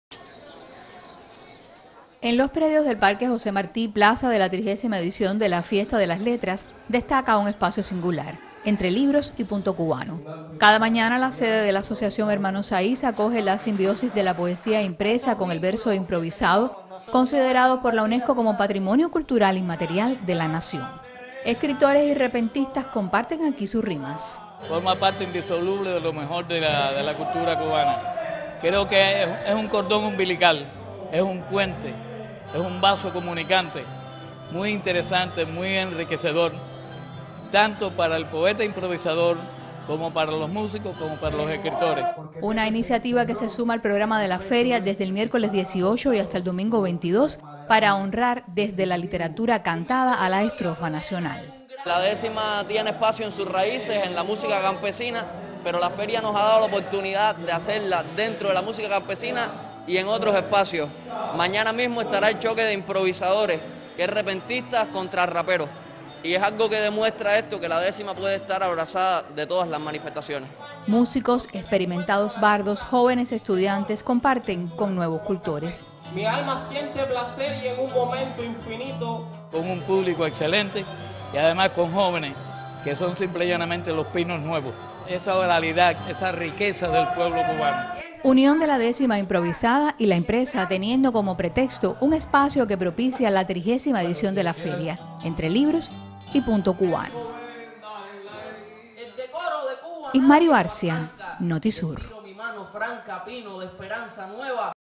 Dedican a la décima espacio en Feria Internacional del Libro en Cienfuegos
El espacio Entre libros y punto cubano, que acogió la sede de la Asociación Hermanos Saíz, es uno de los más concurridos en el programa de la Feria Internacional del Libro en Cienfuegos.